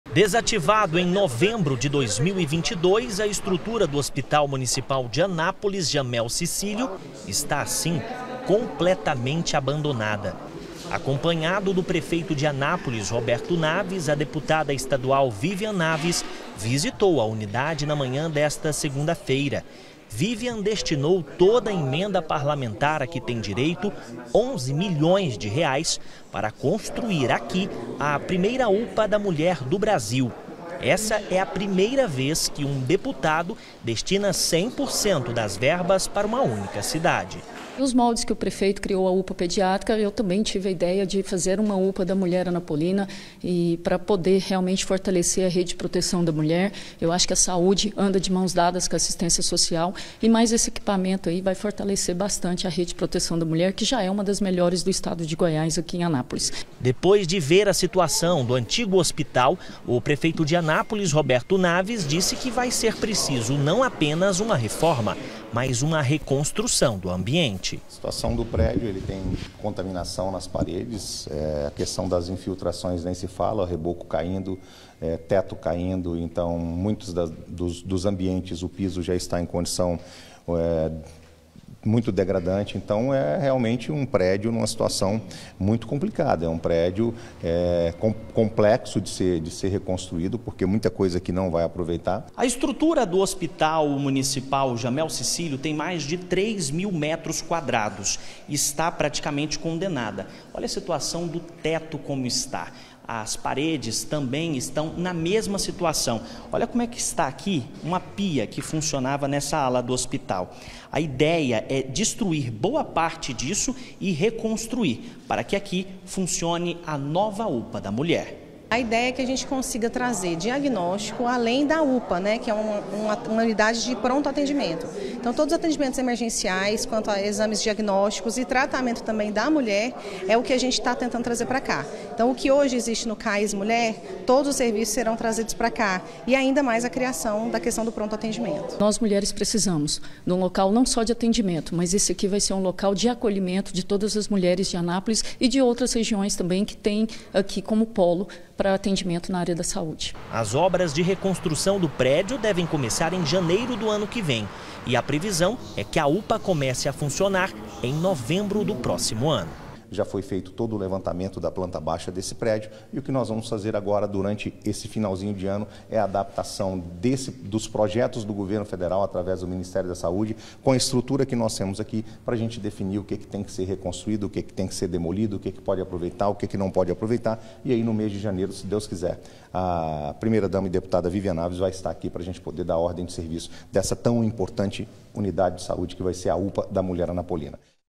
SAÚDE: ANÁPOLIS VAI GANHAR A 1ª UPA DA MULHER DO PAÍS
A DEPUTADA ESTADUAL VIVIAN NAVES VISITOU HOJE O LOCAL ONDE VAI SER CONSTRUÍDA A UPA DA MULHER LÁ EM ANÁPOLIS. ELA DESTINOU 11 MILHÕES DE REAIS PARA CONSTRUÇÃO DA UNIDADE, QUE DEVE SER ENTREGUE NO ANO QUE VEM.